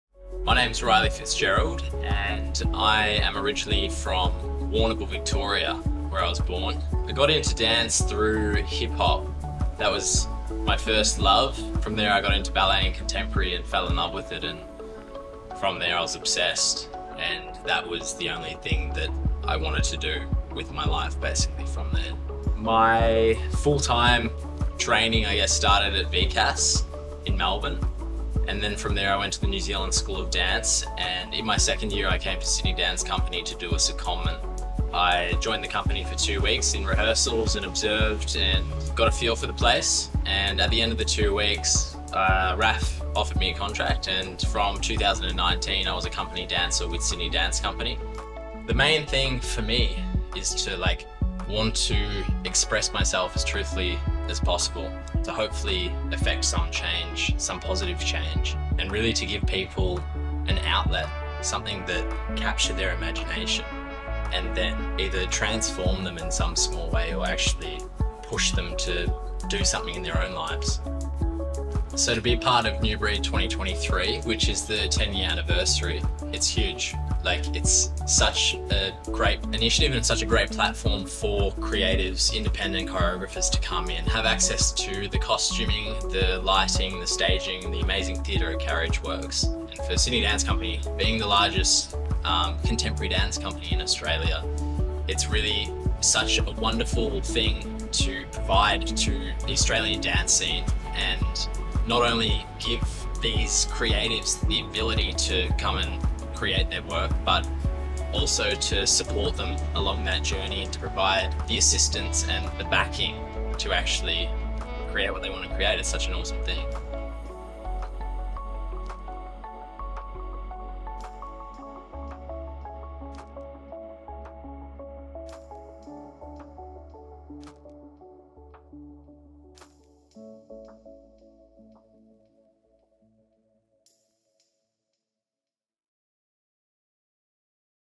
Hear from New Breed 2023 choreographer & Company dancer Riley Fitzgerald.
Hear New Breed 2023 choreographer and Company dancer Riley Fitzgerald’s talk about why he started dancing, what he’s passionate about, and why New Breed is such an important program for emerging artists in the Australian dance scene.

Riley’s work ‘Everyb0dy’s G0t a B0mb’, premieres at Carriageworks in the tenth edition of New Breed from 6 - 16 December.

New Breed
Carriageworks
6 - 16 Dec

📹 Pedro Greig

New Breed is generously supported by The Balnaves Foundation.